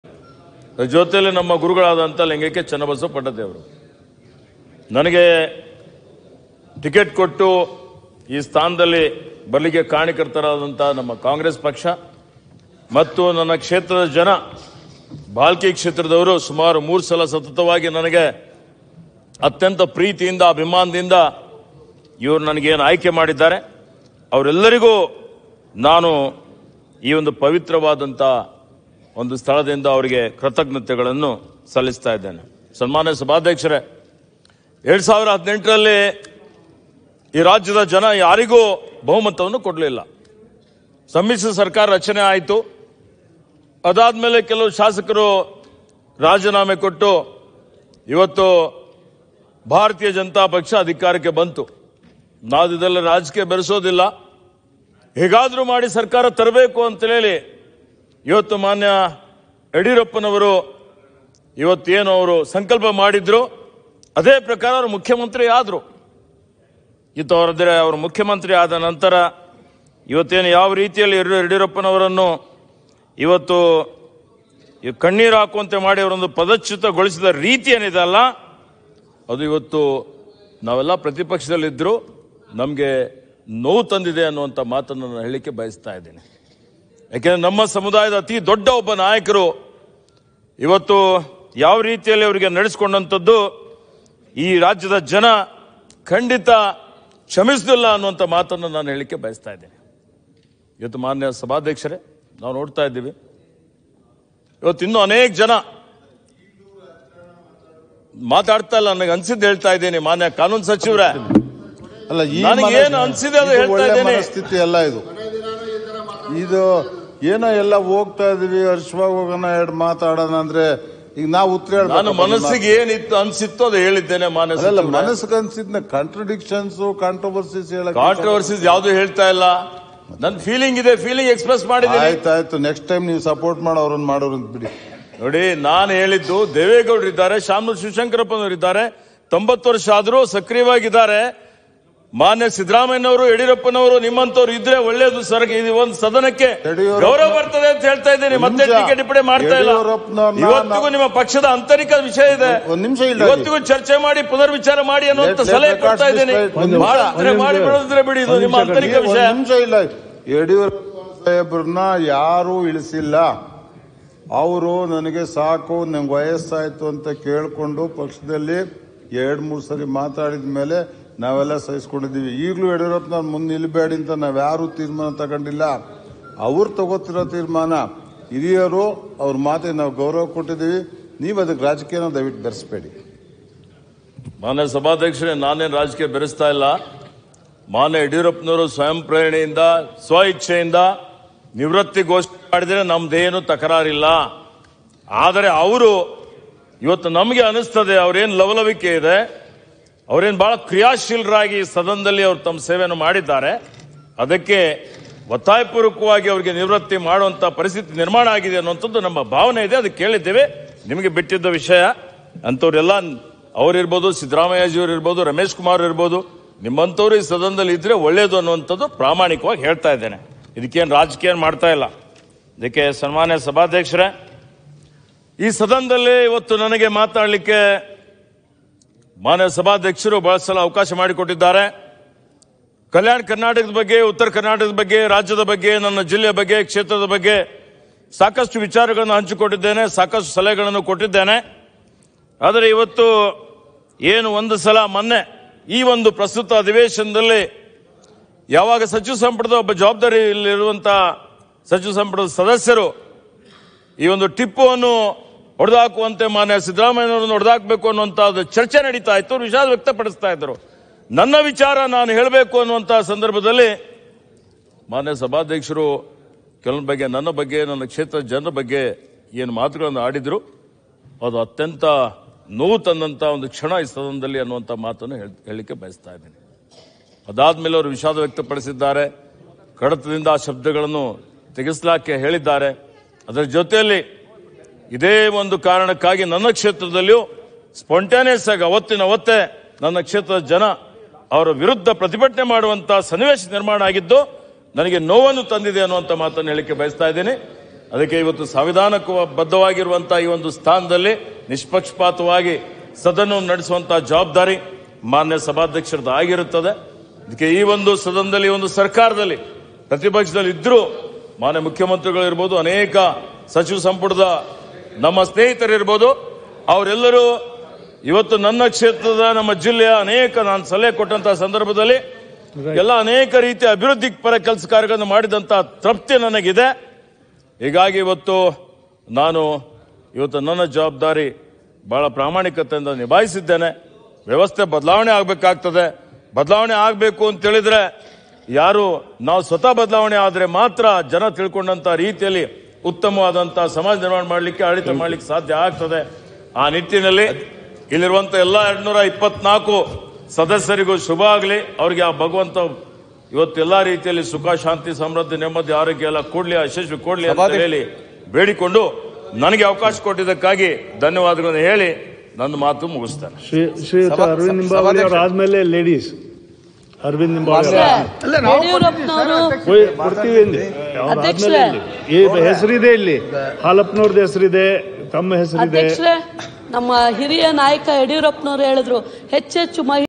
Eshwar Khandre's Speech in Assembly Last Day 2023 | Karnataka Assembly Live | YOYO Kannada News
Karnataka Assembly LIVE | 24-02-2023 | Siddaramaiah | Bommai |HD Kumaraswamy |YOYO Kannada News